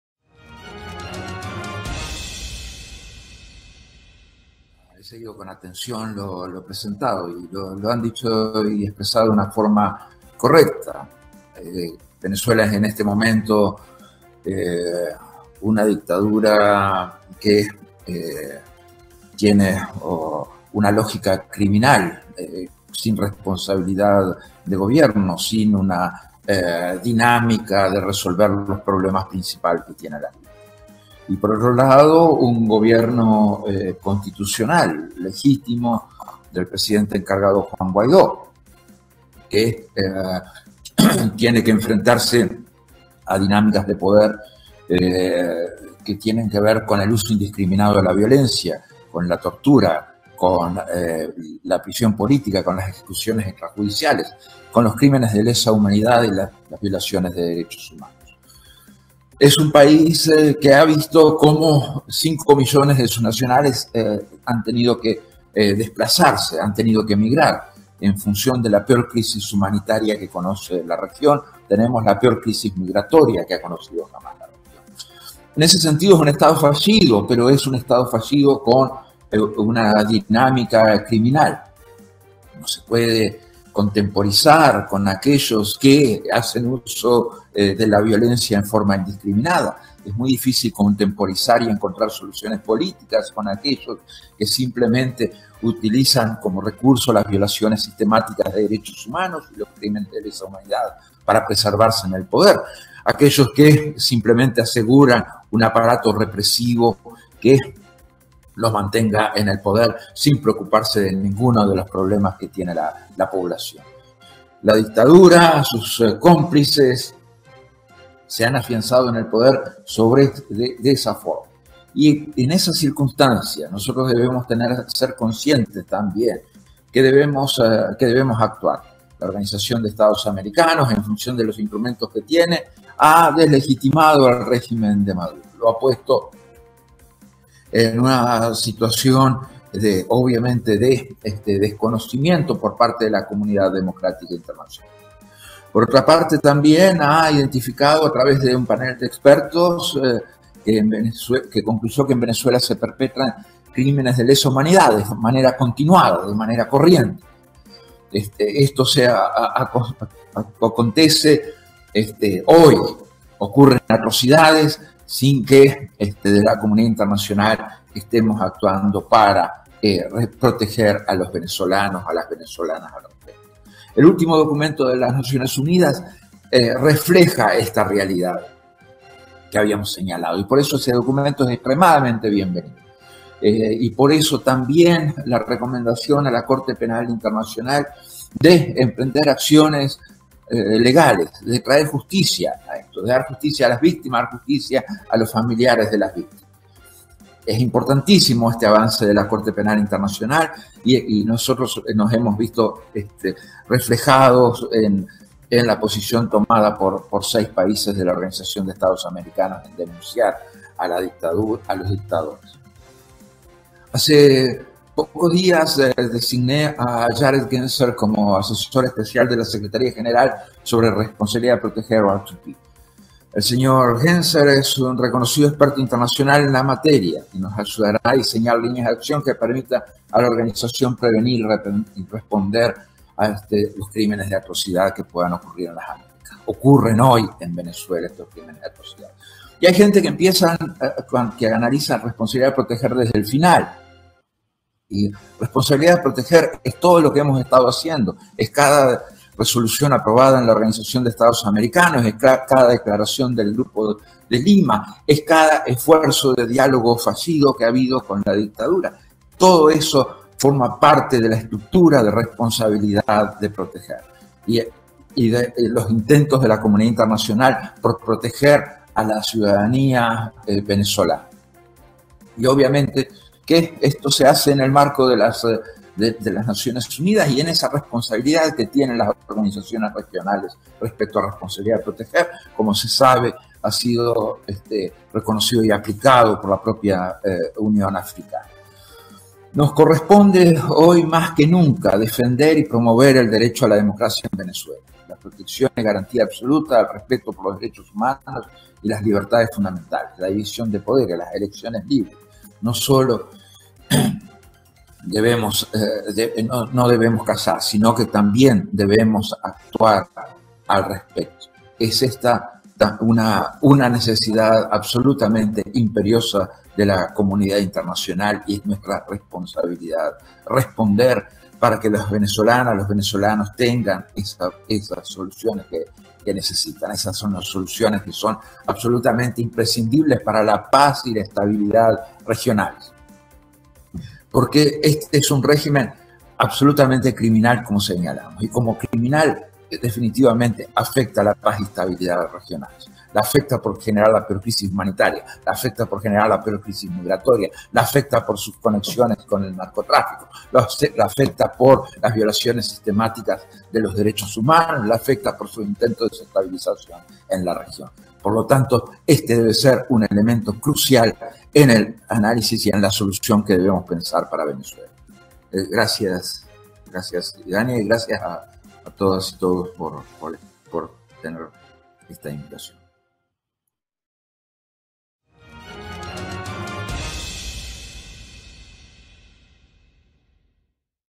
DEBEMOS RECUPERAR LA DEMOCRACIA EN VENEZUELA 19oct LUIS ALMAGRO AL MUNDO
Debemos recuperar la democracia en Venezuela. Es un régimen criminal q afecta la estabilidad regional x generar crisis humanitaria, crisis migratoria, apoya el terrorismo y narcotrafico  y comete crímenes de lesa humanidad